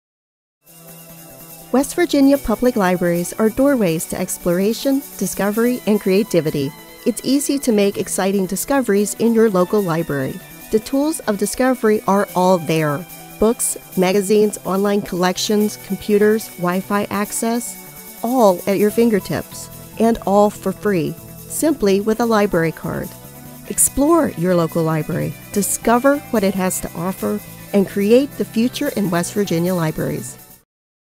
Explore. Discover. Create. "Discover PSA"
West Virginia Public Libraries are a doorway to exploration, discovery, and creativity. Explore your local library… Discover what it has to offer …and Create the future in West Virginia Libraries.